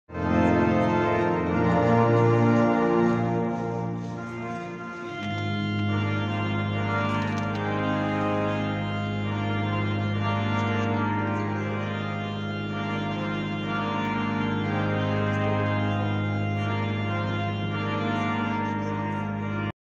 В Полоцке в день Конституции в Софийском соборе 26 юношей и девушек впервые получили паспорта
15 марта в День Конституции в рамках Всебелорусской акции "Мы - граждане Беларуси!" лучшим представителям молодежи, имеющим особые заслуги в учебе, спорте, общественной деятельности, торжественно вручили паспорта. Самый важный документ получили 26 школьников, которым исполнилось 14 лет. Торжественная церемония вручения главного документа по традиции прошла в Софийском соборе.

Для юных жителей Полоцка и района и гостей мероприятия прозвучала музыкальная композиция заслуженной артистки Республики Беларусь Ксении Погорелой.